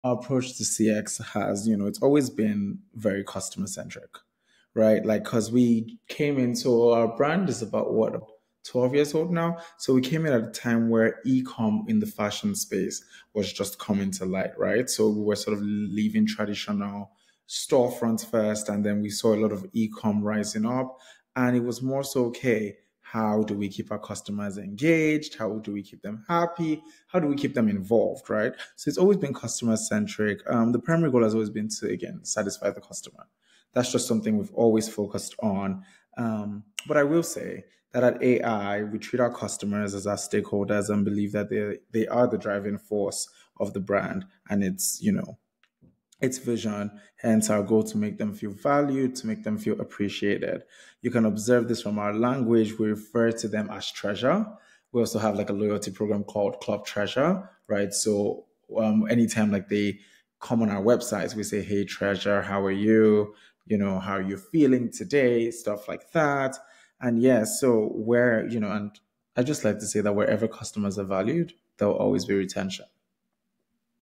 Treat Your Customers as Stakeholders | Ecommerce Roadmap: TJ Balo for Andrea Iyamah
TJ Balo, Head of Customer Relations and Marketing at Andrea Iyamah, emphasizes the importance of a customer-centric approach, detailing how initiatives like Club Treasure and treating customers as a "Treasure" have been key to Andrea Iyamah's success.